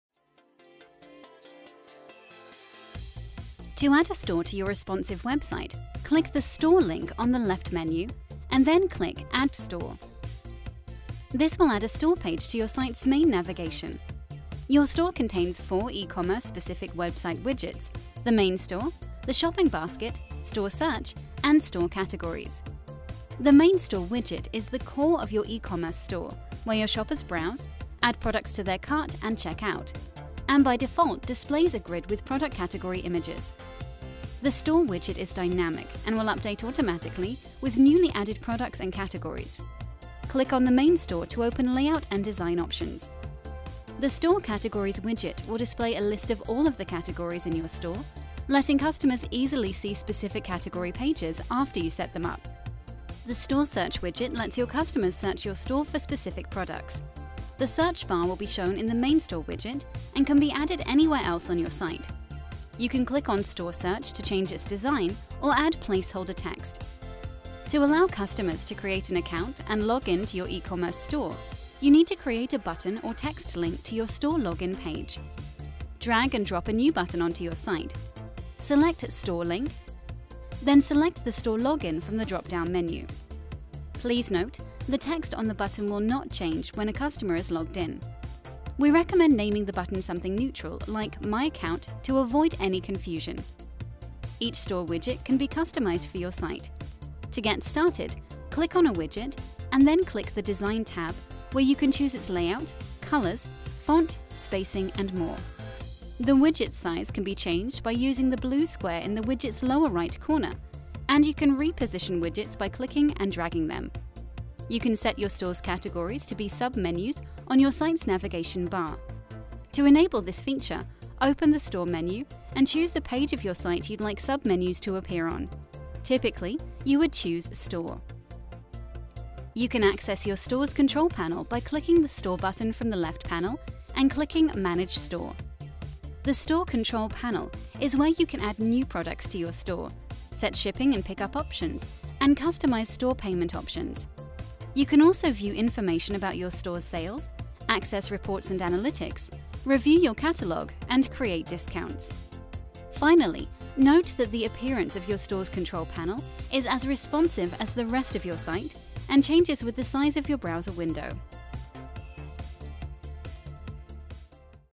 Online Store help: Adding a Store to your Yell Website
Find out how to add an eCommerce Store to your Yell Website. You can also view our help & support articles to answer your eCommerce store questions by clicking